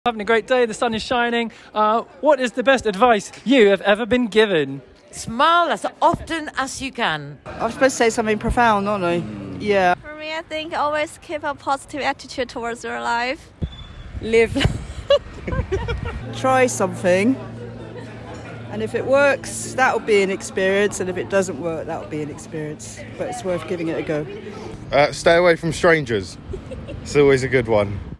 What's the best piece of advice you've ever been given?
What's YOUR New Year's Resolution? Here's some inspo from the streets of Brighton UK interviews street strangers ukcomedy impo wisdom brighton tomveryzer